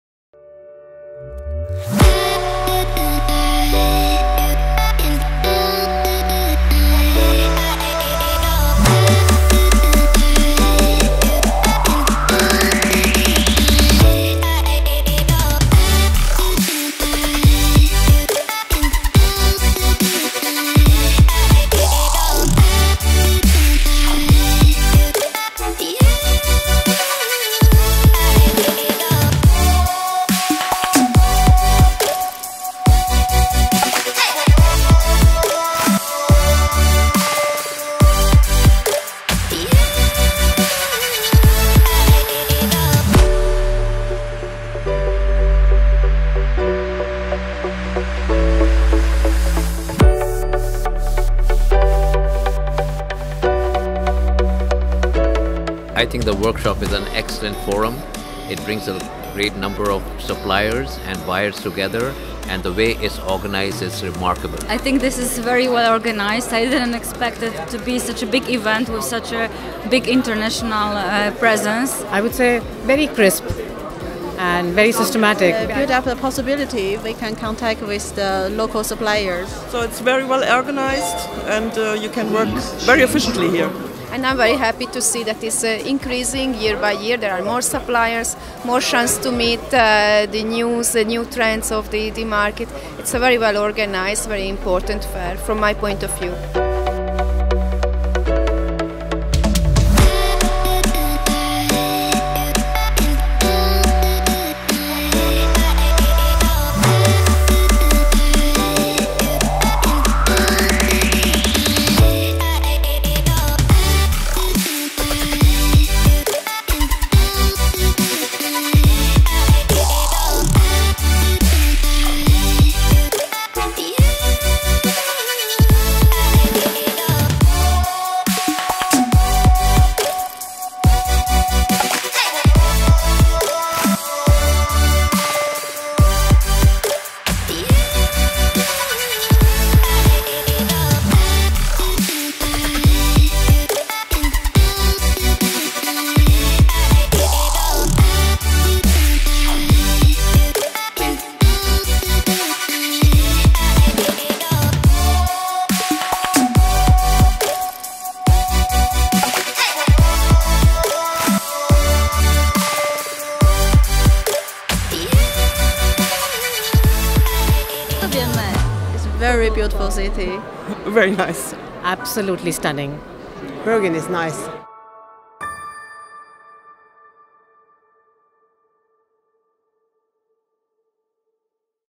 Norwegian Travel Workshop 2017 Bergen
Bergen was the host of the Norwegian Travel Workshop (NTW) in 2017.